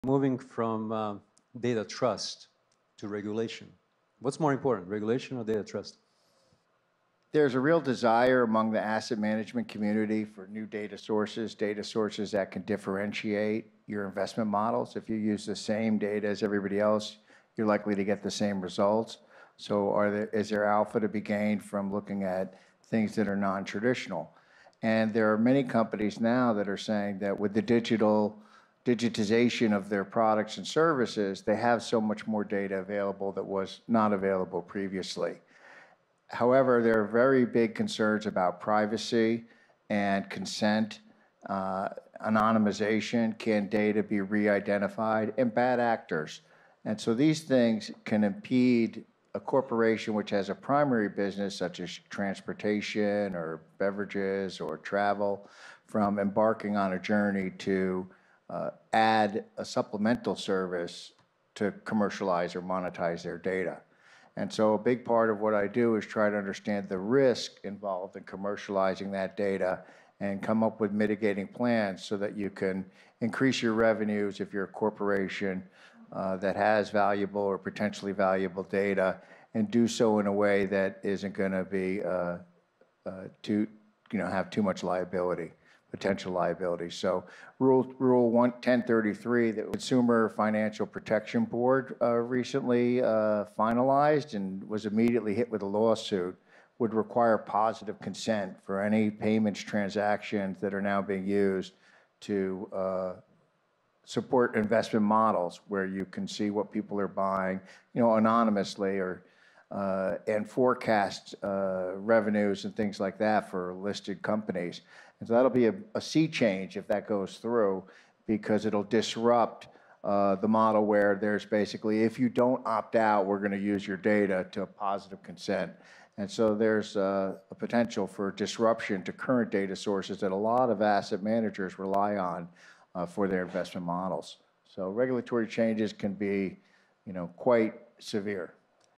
"Concerns about Privacy and Consent often impede many companies to monetize their proprietary data"
Moderator
Vidak Radonjic - Managing Partner, The Beryl Consulting Group LLC
Speakers:
Tom Grant - VP Research, Apptopia Inc.
Sarah McKenna - CEO, Sequentum
Joseph Sommer - Managing Director, EY
Pamela Munger - Lead & Senior Market Analyst, Vortexa

"Balancing data monetization with privacy risks is critical as new regulations reshape investment models."

PANEL - Alternative Data - Trends, Sectors, and Monetization: New sectors are emerging and international markets are opening up. Trends in monetization. Where can competitive advantages be found. The growing use of alt data by PE and VC, and how due diligence is transforming.

- The panel discussed the growing importance and applications of alternative data, particularly in hedge funds and private corporations, focusing on how it can generate competitive advantages and improve productivity.

- Key points included the evolving nature of mobile app data, the challenges in collecting high-quality web-scraped data, regulatory impacts on data providers, and the use of satellite and customs data for tracking seaborne cargoes.

- The conversation also covered the future outlook for alternative data usage across various sectors, emphasizing innovation, responsible AI practices, and integrating unstructured with structured data to enhance decision-making processes.

Transcript: Moving from data trust to regulation, what's more important, regulation or data trust? There's a real desire among the asset management community for new data sources, data sources that can differentiate your investment models. If you use the same data as everybody else, you're likely to get the same results. So is there alpha to be gained from looking at things that are non-traditional? And there are many companies now that are saying that with the digital digitization of their products and services, they have so much more data available that was not available previously. However, there are very big concerns about privacy and consent, anonymization, can data be re-identified, and bad actors. And so these things can impede a corporation which has a primary business such as transportation or beverages or travel from embarking on a journey to add a supplemental service to commercialize or monetize their data.

And so a big part of what I do is try to understand the risk involved in commercializing that data and come up with mitigating plans so that you can increase your revenues if you're a corporation that has valuable or potentially valuable data and do so in a way that isn't going to be too have too much liability, potential liability. So, rule 10,333, the Consumer Financial Protection Board recently finalized and was immediately hit with a lawsuit, would require positive consent for any payments transactions that are now being used to support investment models where you can see what people are buying anonymously and forecast revenues and things like that for listed companies. And so that'll be a sea change if that goes through because it'll disrupt the model where there's basically, if you don't opt out, we're going to use your data to achieve positive consent. And so, there's a potential for disruption to current data sources that a lot of asset managers rely on, for their investment models. So, regulatory changes can be quite severe.